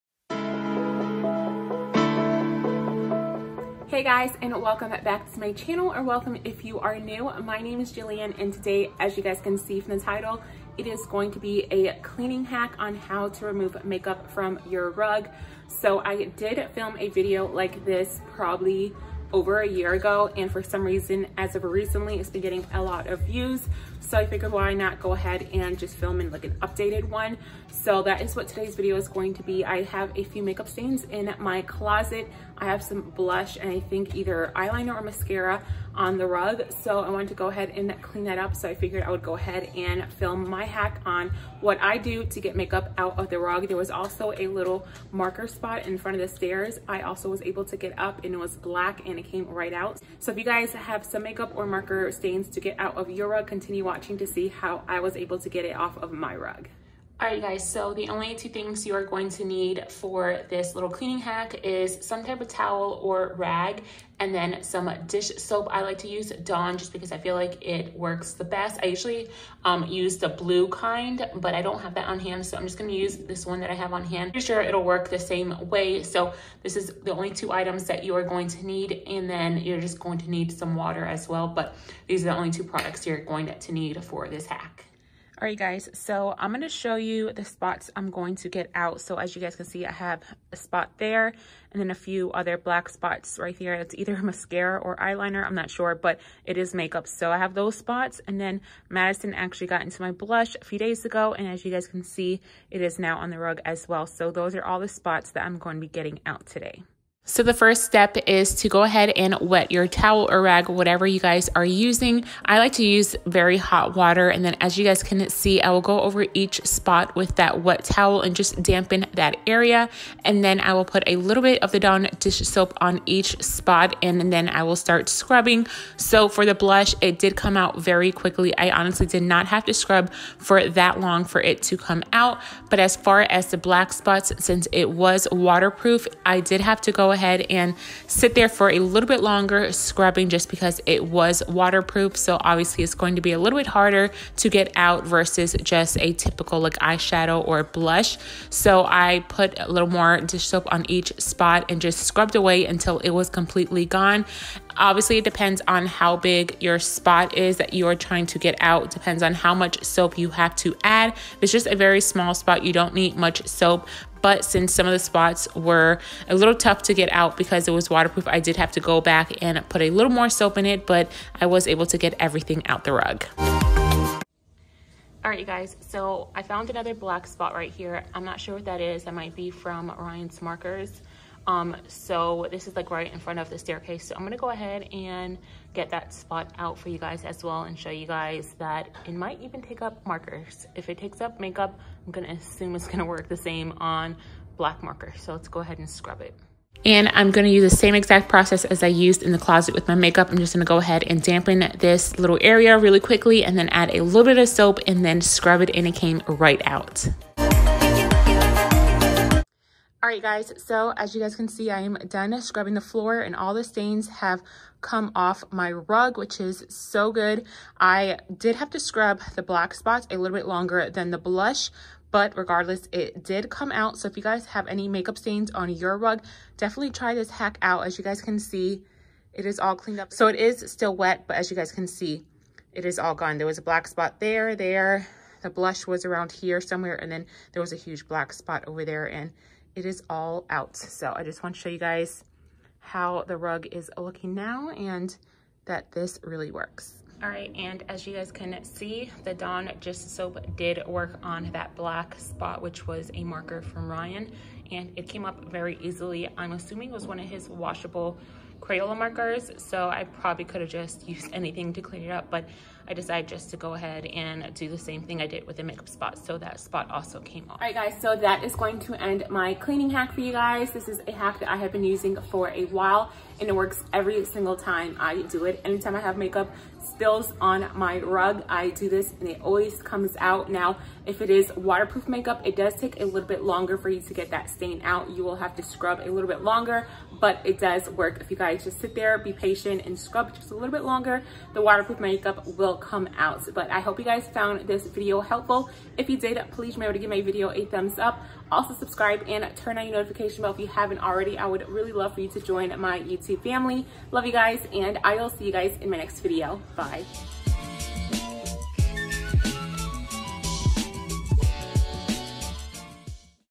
HOW TO REMOVE MAKE-UP FROM CARPET | CLEANING HACK
Hey friends! Today I have a quick cleaning hack for you. I’m
Sharing with you guys what I do to get makeup stains out of my carpet because it happens often especially with the kids. It’s such a easy and quick hack, that I thought you guys might be able to use yourself.

JillianS502

Filming Equipment

Music credit